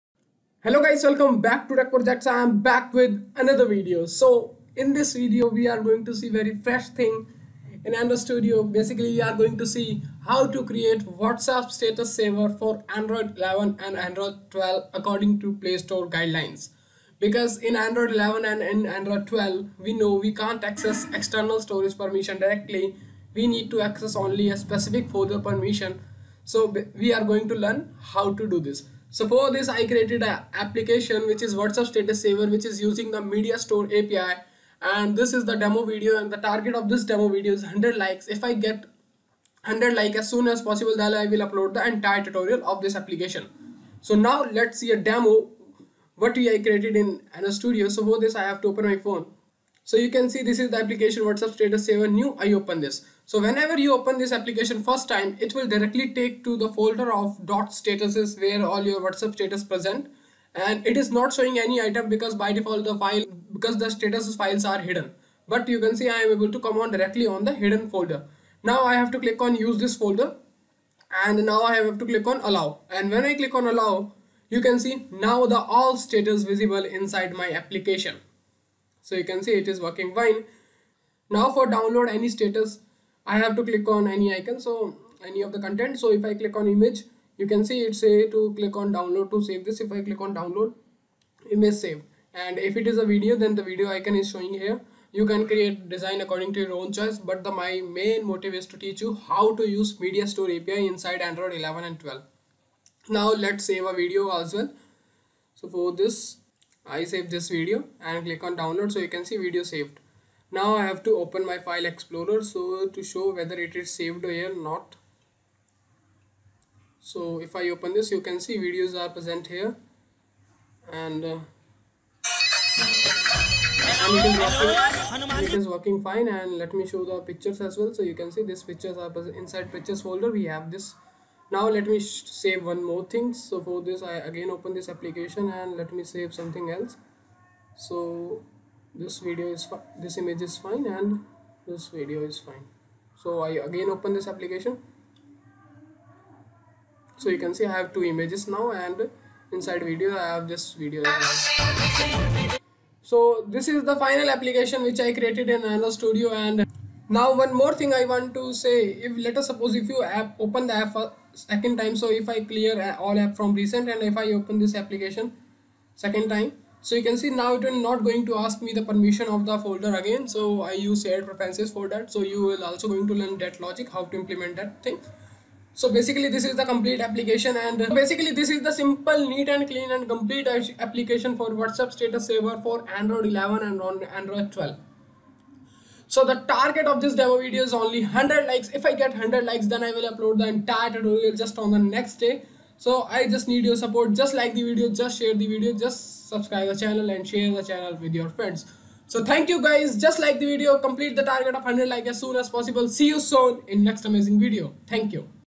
What's app status saver app using Media Store API | For Android 11 & 12 | For Playstore | Demo video
In this video we are going to see DEMO Video of whatsapp status saver application for android 11 and android 12

If you think that I  used your property by mistake in my videos then I'm Ready to give you whole credit
[For Source codes visit here]

whatsapp status saver for android 11 & 12
how to create whatsapp status saver for android 11 & 12